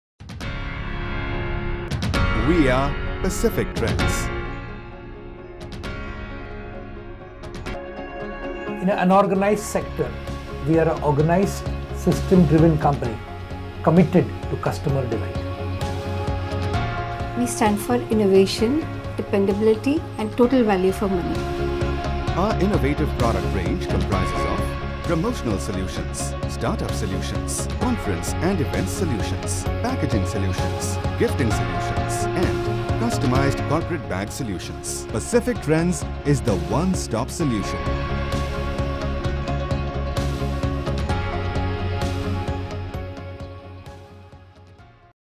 Pacific Trends MD Video (Motion Magic Media)
This is a corporate video made by Motion Magic Media for one of its client Pacific Trends. Video contains Motion Grpahics and infographics. The video shows Pacific Trends MD Mr. Arunaagiri speak about the company.

Motion Magic Media has produced corporate films & marketing videos for many business owners, who understand and appreciate the potential of a business video.

Category:
Film & Animation